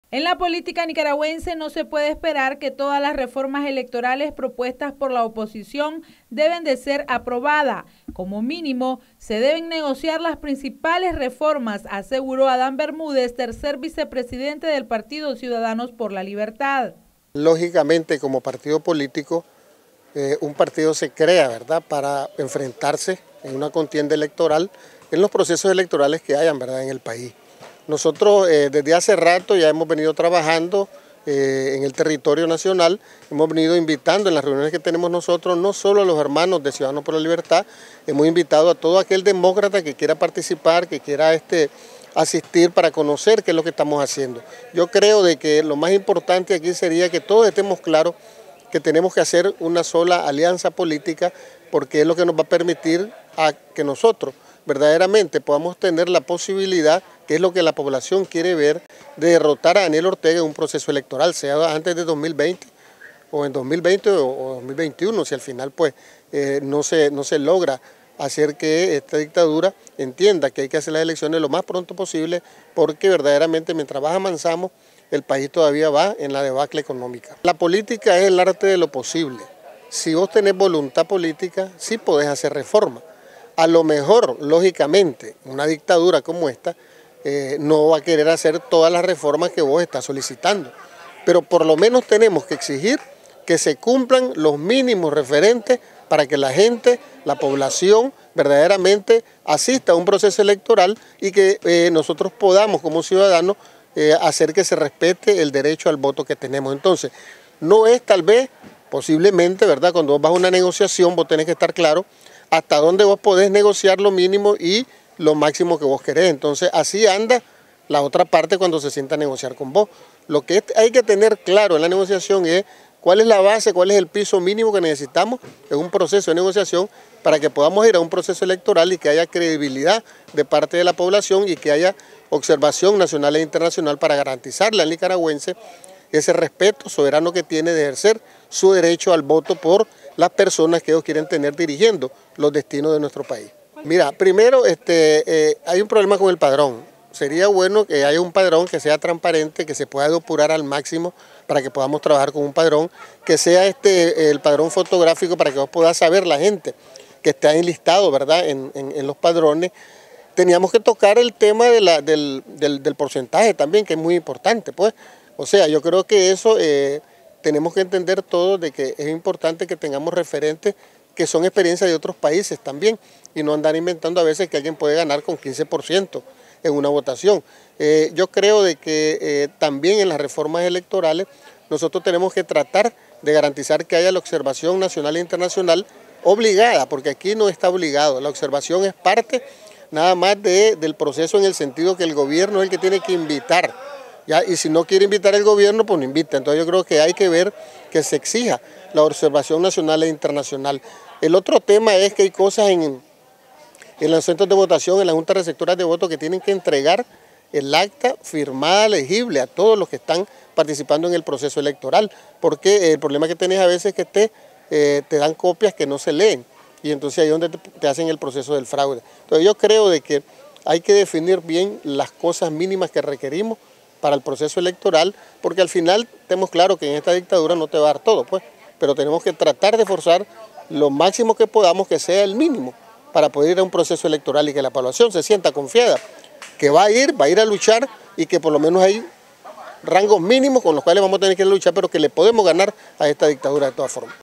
“SE DEBEN NEGOCIAR POR LO MENOS LAS REFORMAS ELECTORALES PRINCIPALES”
Hay que garantizar en una negociación por lo menos las reformas electorales principales, dijo Adán Bermúdez miembro del Partido Ciudadanos por la Libertad.